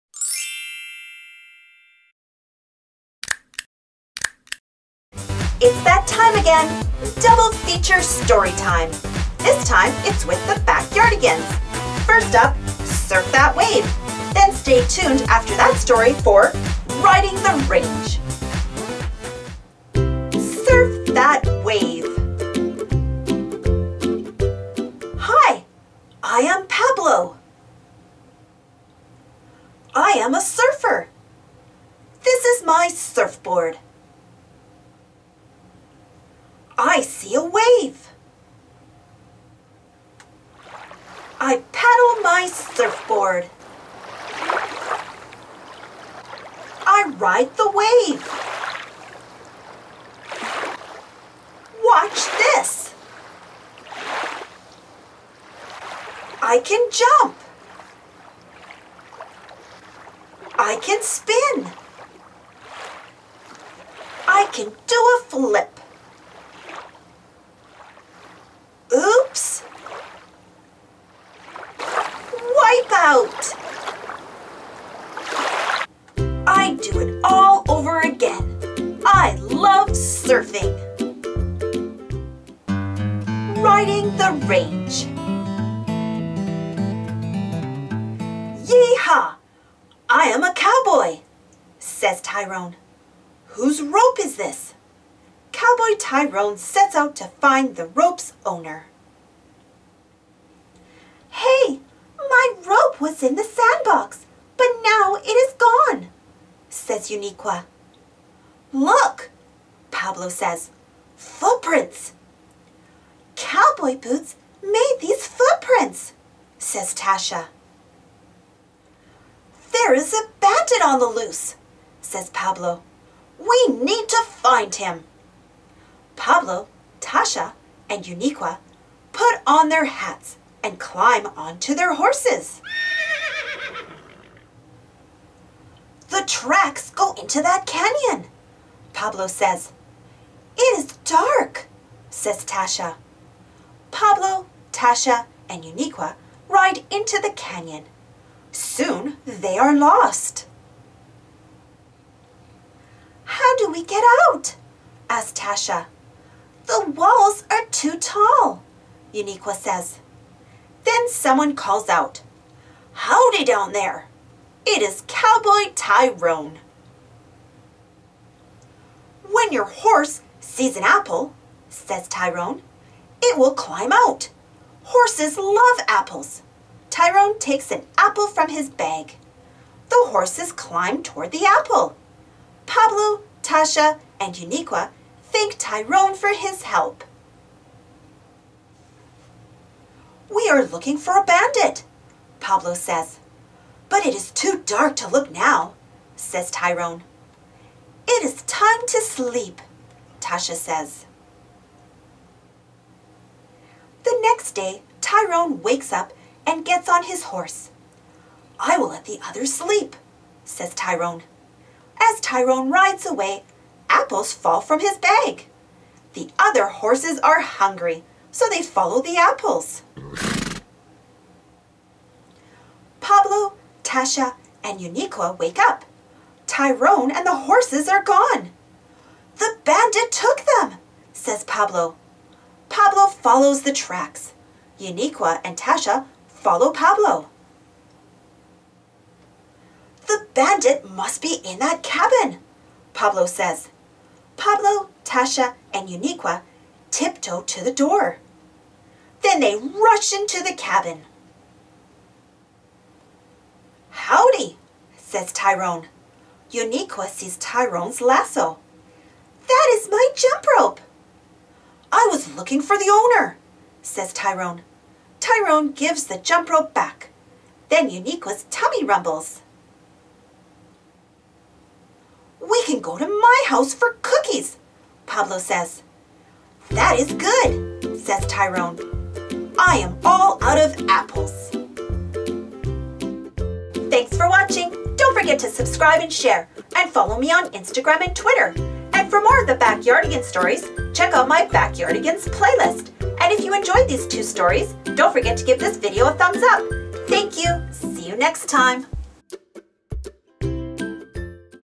THE BACKYARDIGANS "SURF THAT WAVE" & "RIDING THE RANGE" - Read Aloud | Storybook for kids, children
*DOUBLE FEATURE!*  2 "The Backyardigans" stories! - Surf That Wave & Riding the Range
Both are great early reader books. Read along or sit back and enjoy the stories!

A new story 6 days a week! Join me!

If you enjoyed this story and my channel please share it with a friend and give it a "thumbs up"! Thank you.

Based on the TV series "The Backyardigans" as seen on Nick Jr.